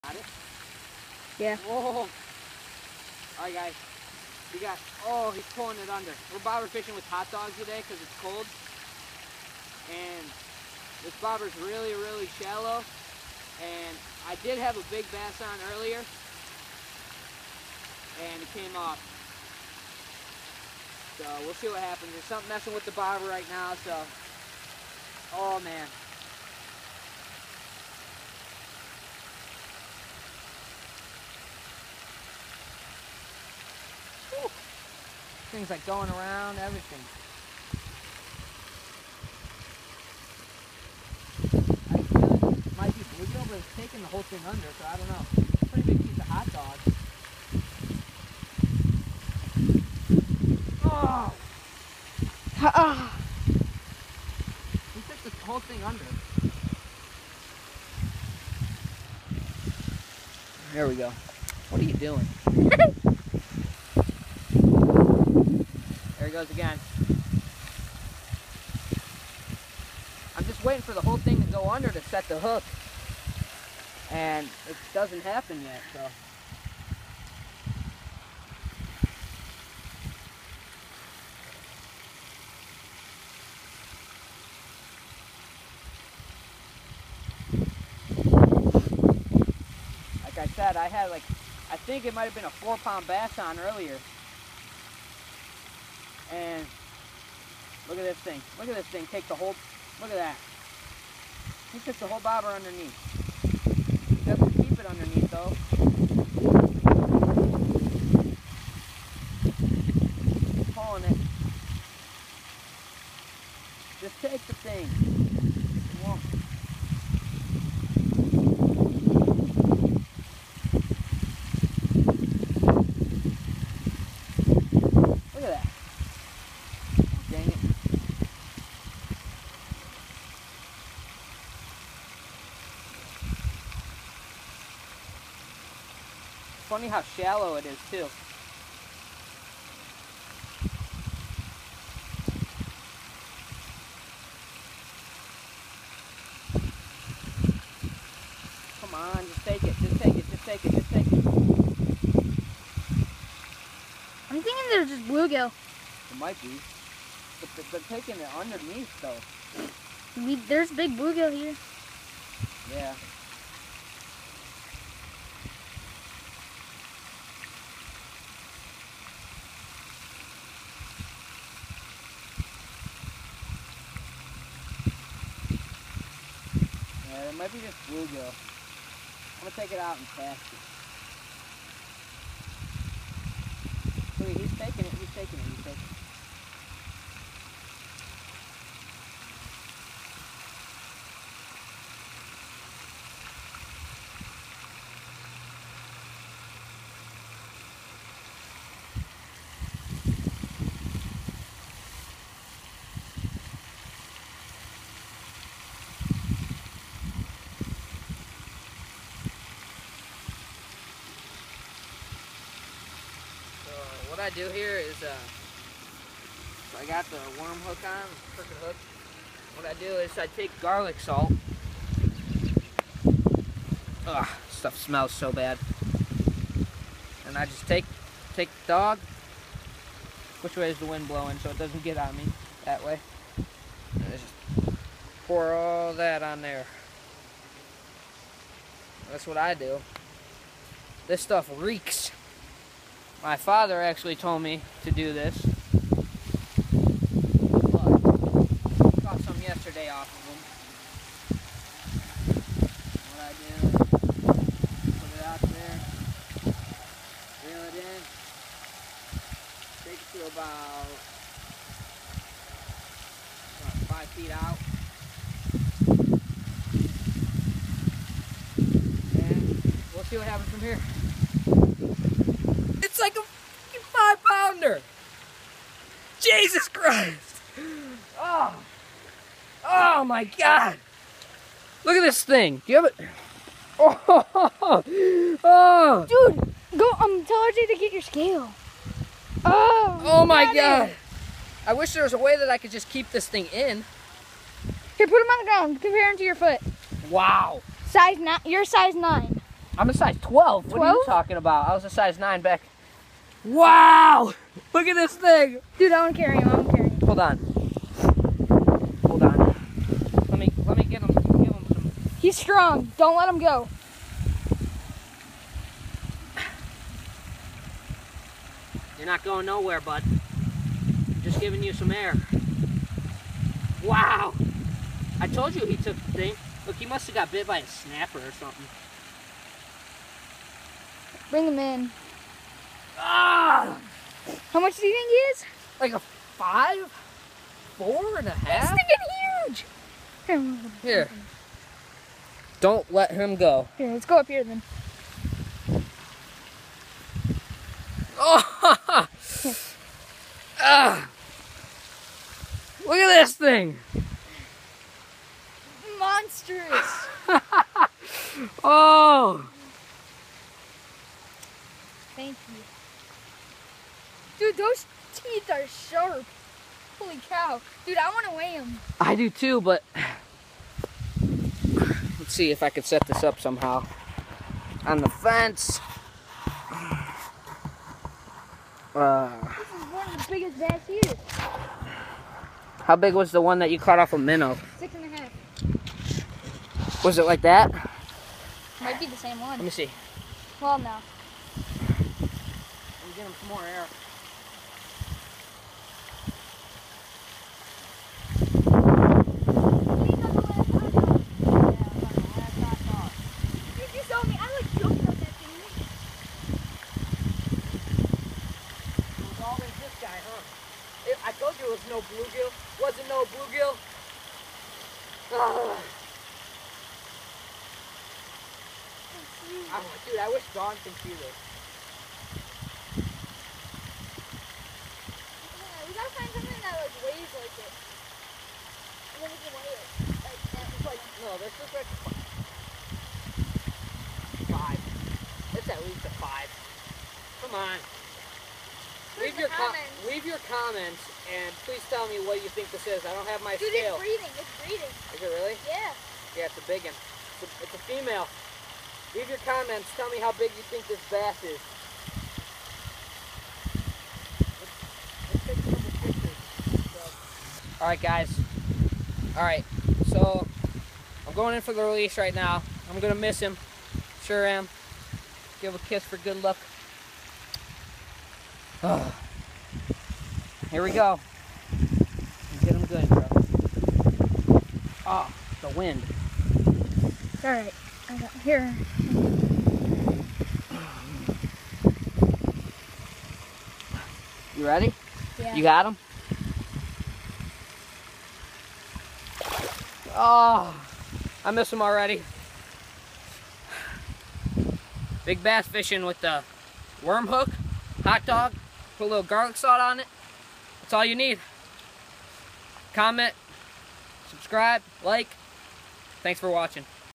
Bass fishing with hotdogs
I learned that hot dogs work on windy and cold days.